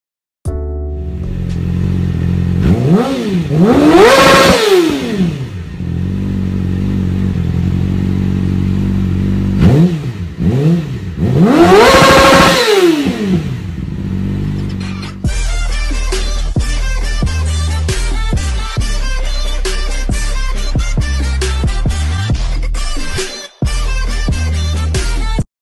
2013 YAMAHA YZF R6 with Akrapovič Full Exhaust System Sound
This is the sound of Yamaha R6 2013 with Akrapovič Exhaust Full System. Listen the sound and see the exhaust flames. Angry bike 💨

We are a SPORTBIKES TEAM from North Macedonia. Our name is MOTO TETOVA TEAM. To see more videos from us go👇🏻